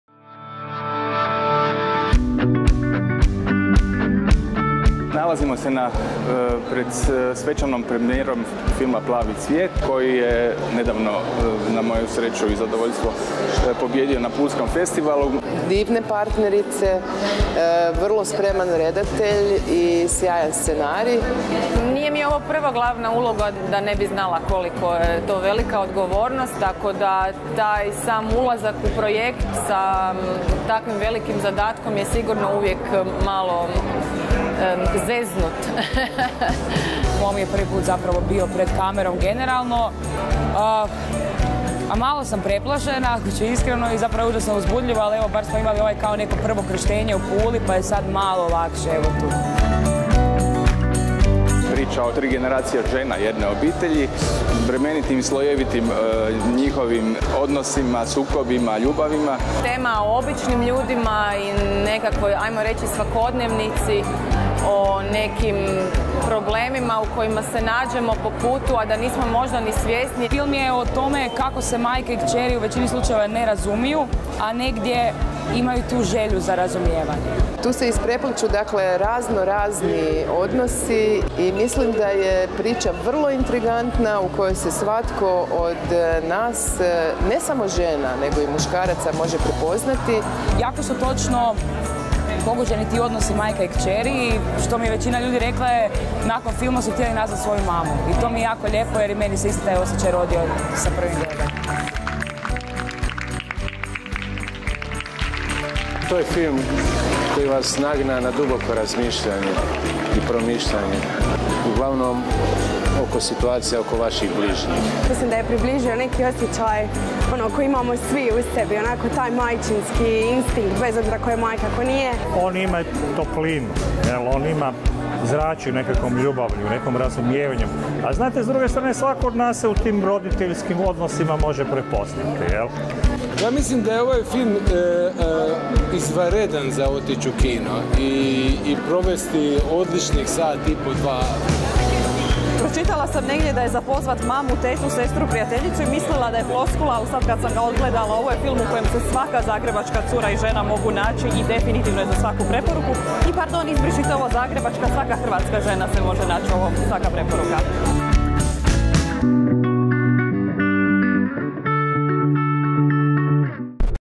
Svečana premijera filma 'Plavi cvijet'
'Plavi cvijet', novi film Zrinka Ogreste, imao je svoju svečanu premijeru 12. listopada 2021. u Kaptol Boutique Cinema.  Ekipu filma predvođenu glavnim glumicama Vanjom Ćirić, Anjom Šovagović-Despot i mladom Teom Harčević, podržali su brojni  ljubitelji domaćeg filma.